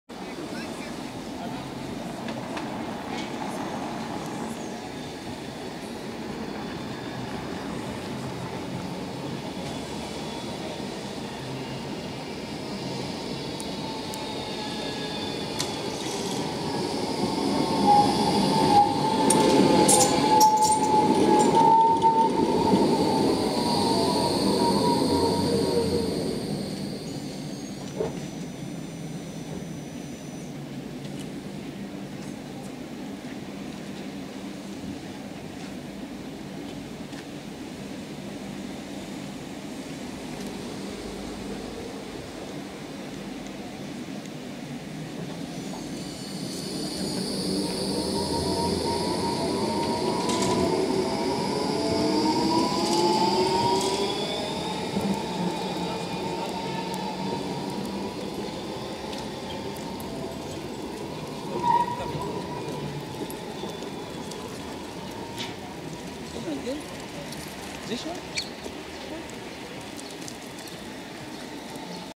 Straßenbahn Frankfurt am Main in Zoo
VGF S-Wagen der Linie 14 vor der Zoo.
14系統の路面電車がフランクフルト動物園停留所に発着する。